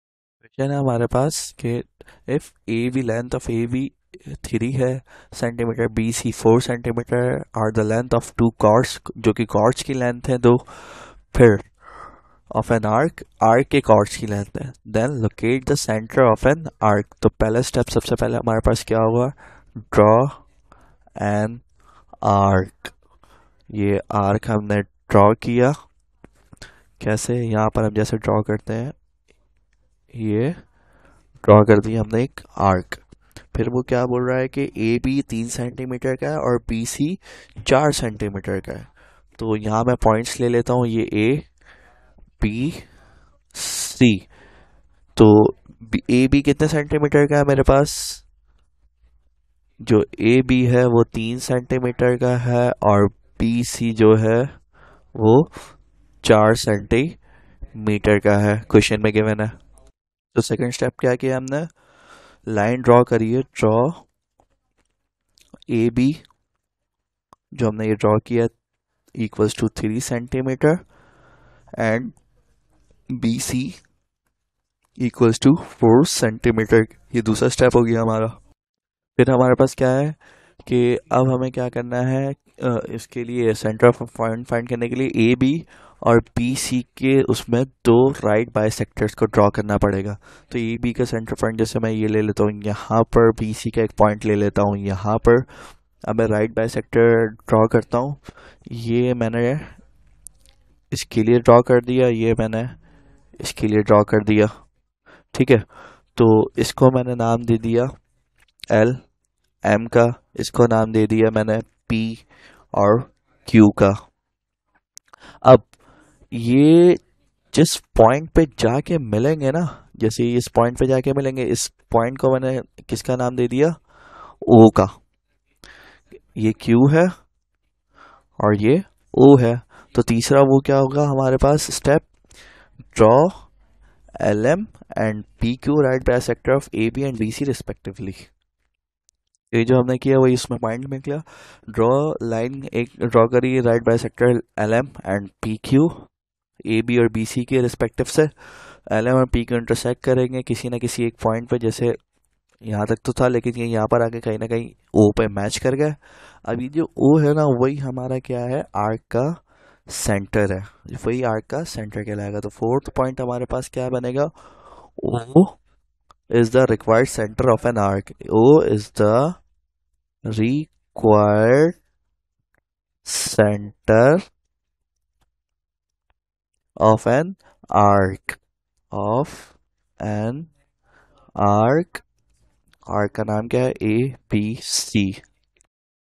Class X Math Chapter 13 - Practical Geometry Circles Ex 13.1 Question 3 Part 1
Prepare for your matric exams with Class 10 Practical Geometry Circles playlist. This video covers key concepts and ideas for Class 10 Math Chapter 13 to ensure you do well in your matric exams.